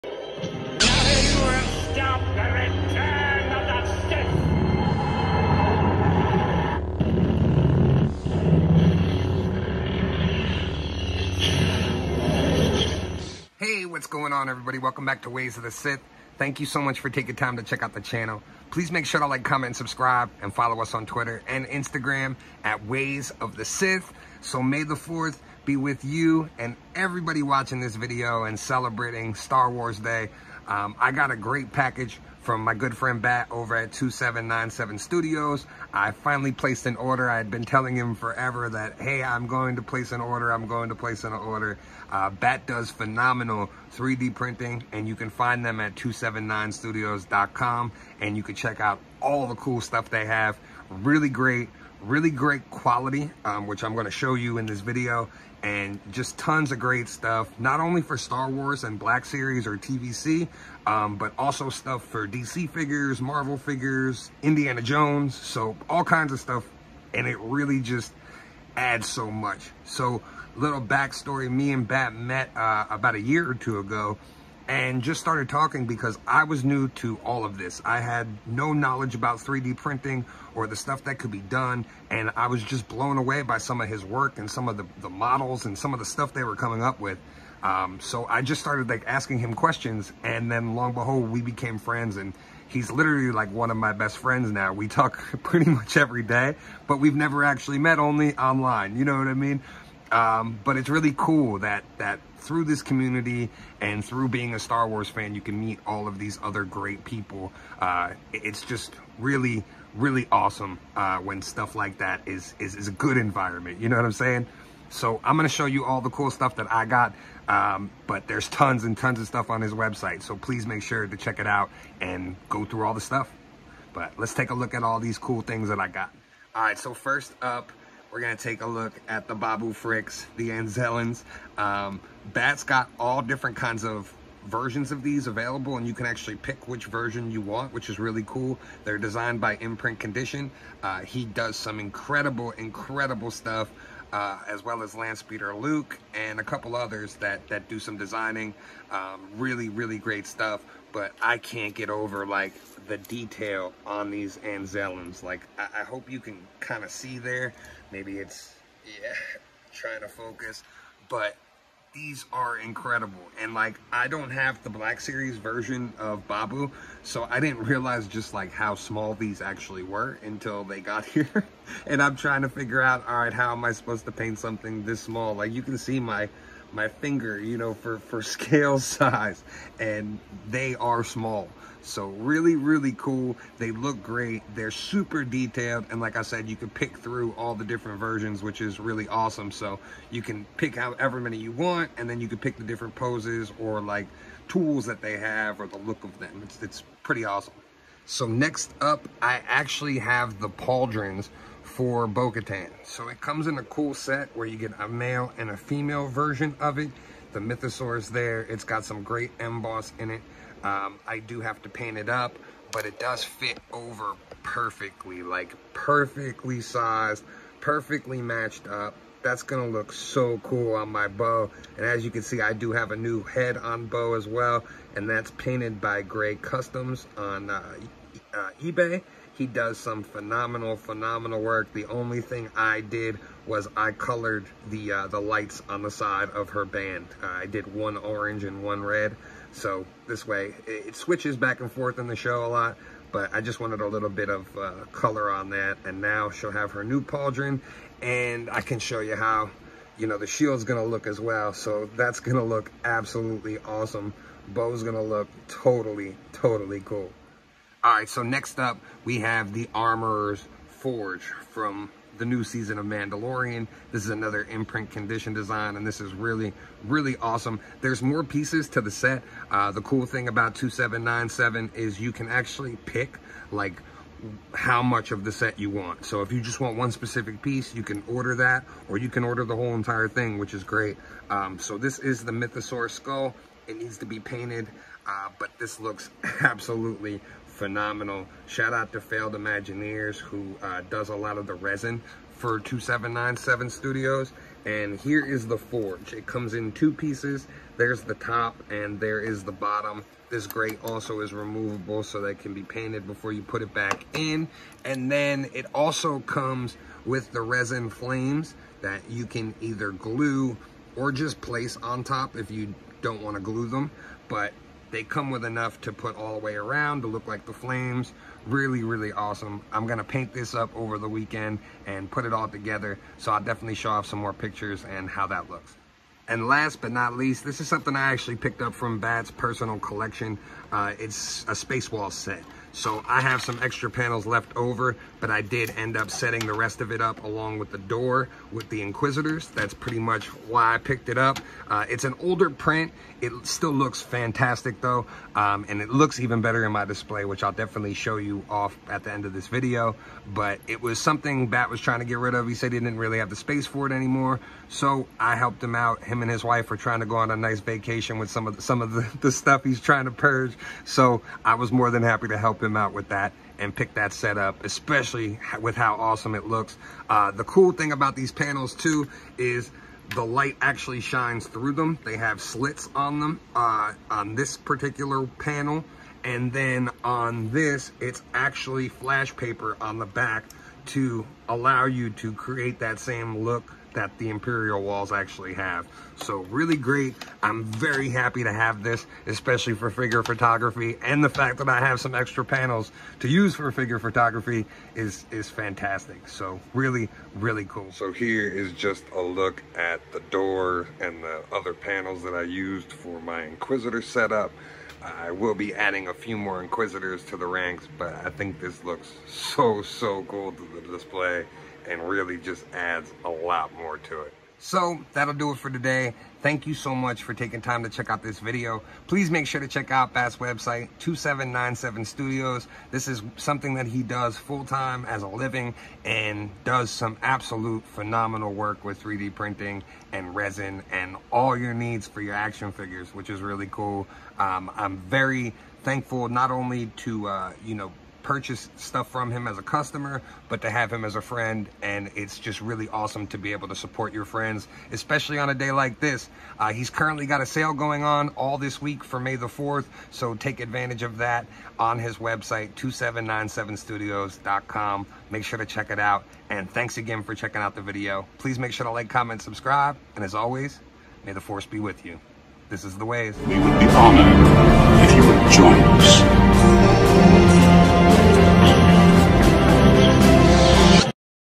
Star War Black Series 3D Print Haul From 2797Studios
Welcome to @WaysOfTheSith thanks for checking out the channel! In this video I show off some 3D Printed Star Wars Black Series Items that I purchased from 2797Studios including The Armorer’s Forge, Bo Katan’s pauldron, Anzelians, & More. Please make sure to check out the video and make sure to check out 2797studios.com